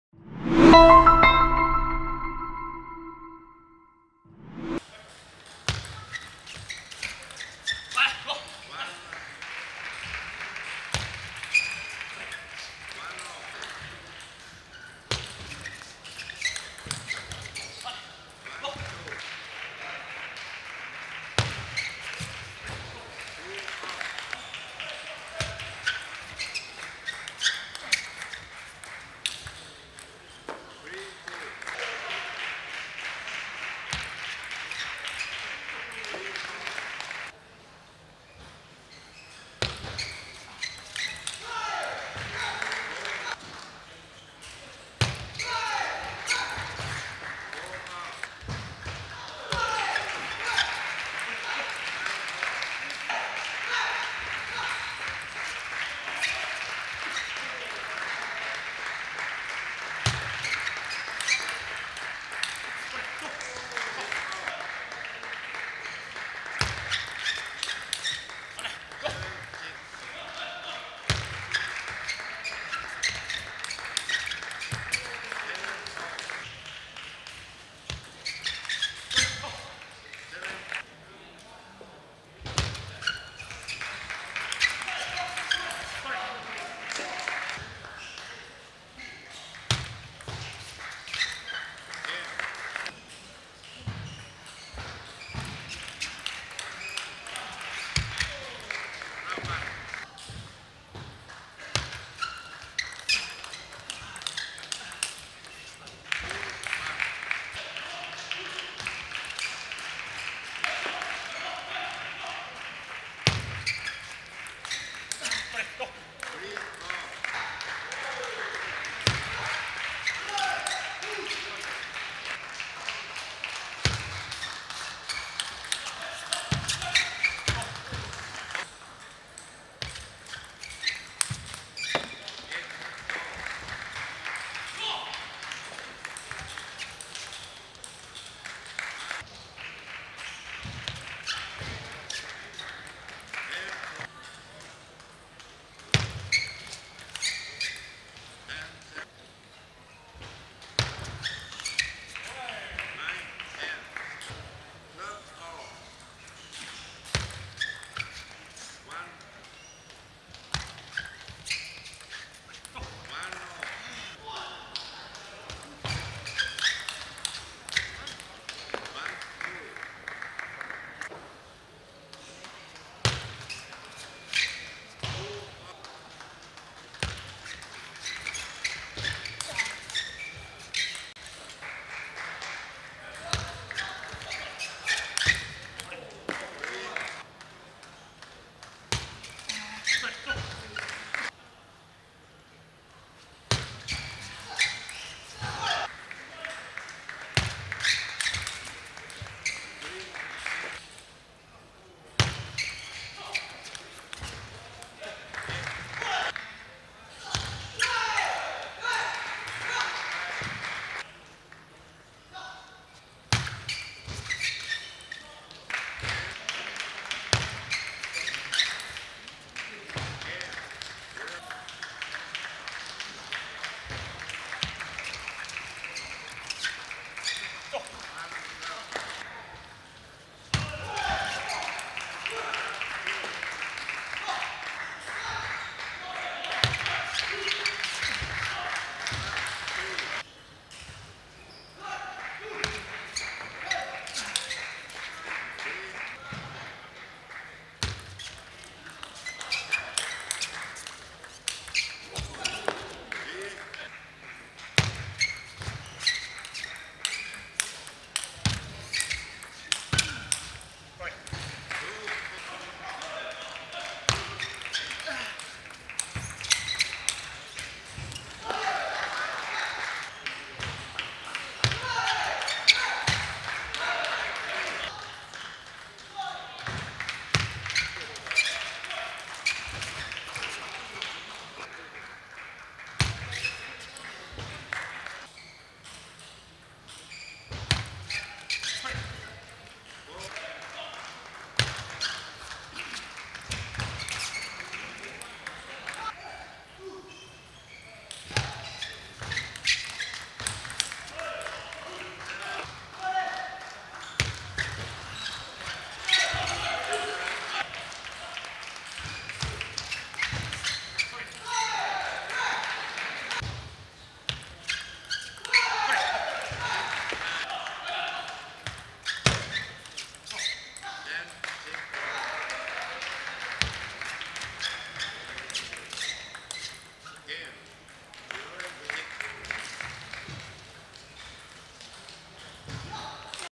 Jin Ueda vs Beh Kun Ting | 2019 ITTF Challenge Thailand Open (R64)
Review all the highlights from the Jin Ueda vs Beh Kun Ting the Seamaster 2019 ITTF Challenge, Thailand Open.